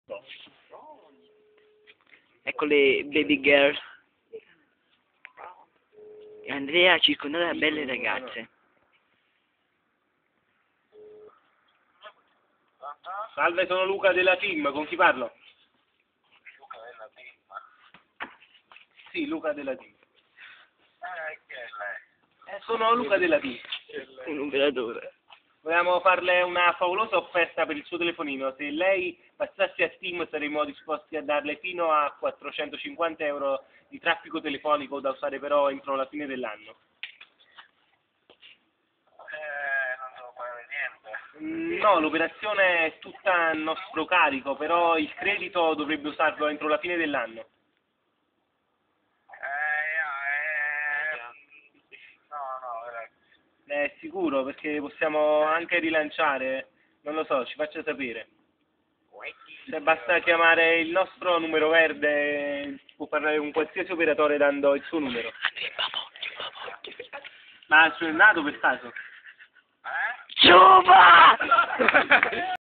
Cannello - " offerta vantaggiosa"
Qui cannello si mette nei panni di un operatore della tim e propone un'offerta VANTAGGIOSISSIMA , ma succede qualcosa di inaspettato.  VEDETE TT I VIDEO DI CANNELLO GIRATI A CASAMARCIANO  by MANZI ANTONIO